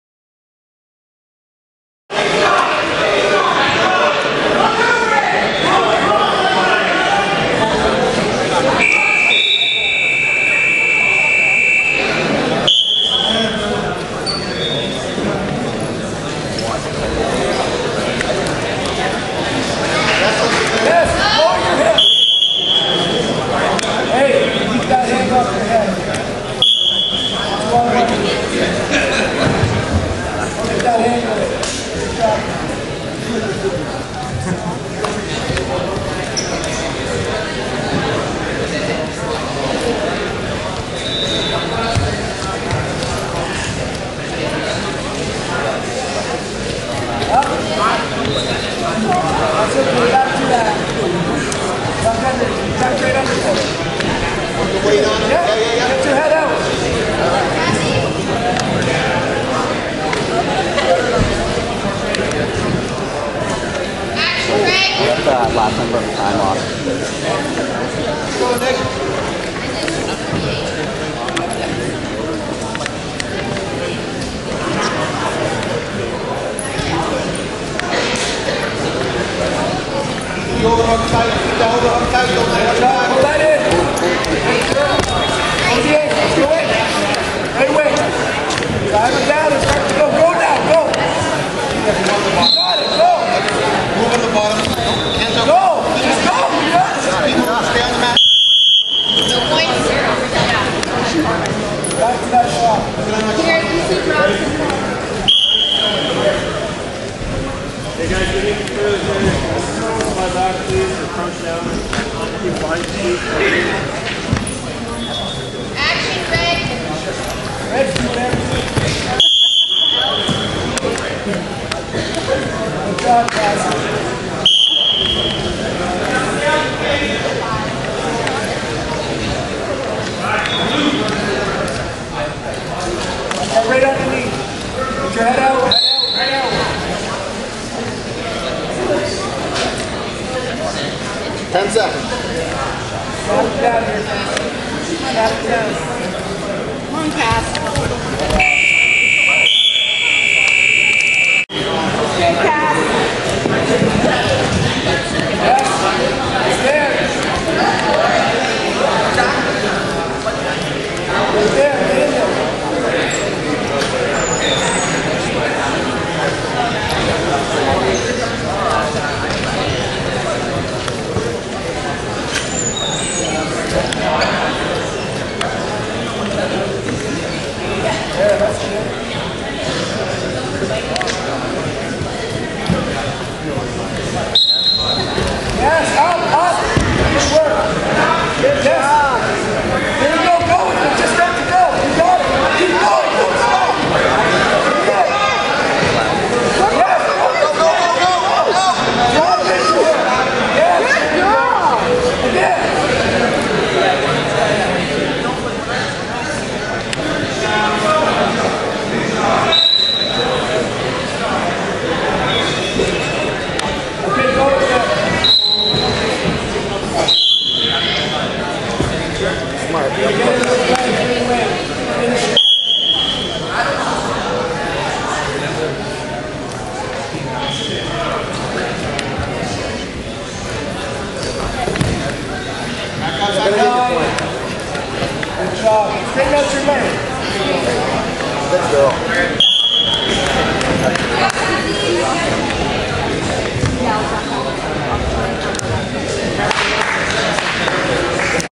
2013 Ontario Junior Championships: 48 kg Cassie Leigh vs. Nikki Fraser
2013 Ontario Junior Championships 48 kg Match Between Cassie Leigh (London-Western Wrestling Club) and Nikki Fraser (Guelph Wrestling Club)